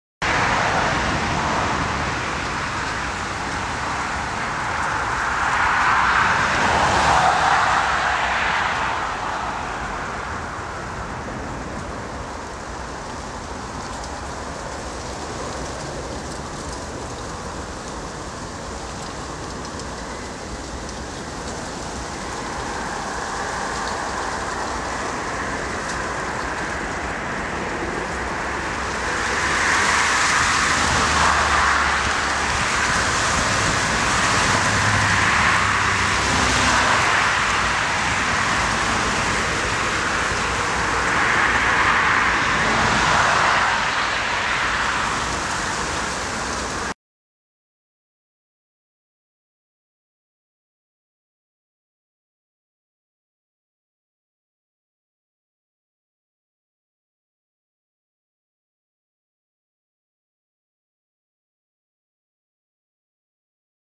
Bird sitting on the runway // CRJ-900 crosswind landing.
You should watch this in full screen.

Have a look at this bird relaxing on the active runway while a small regional plane arrives on runway 23R. (Eurowings Canadair CRJ-900)